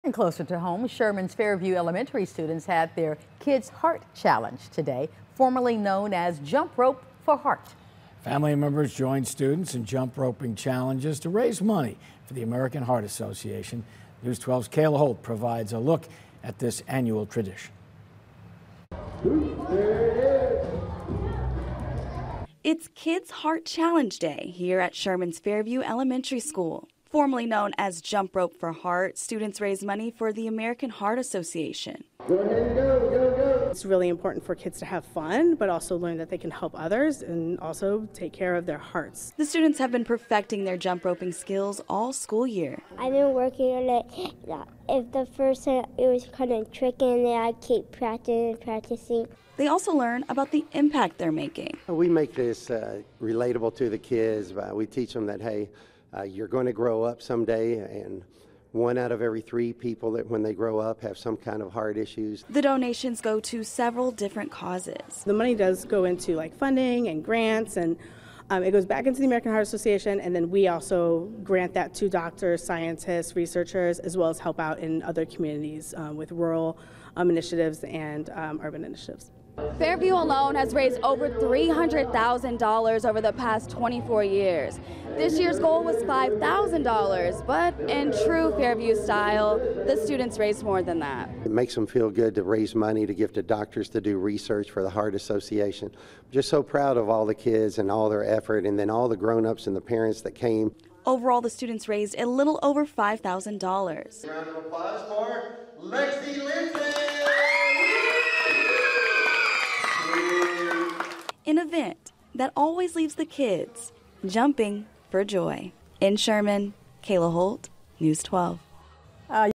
Fairview Elementary students participate in American Heart Association’s ‘Kids Heart Challenge’
Fairview Elementary students jump rope for a good cause.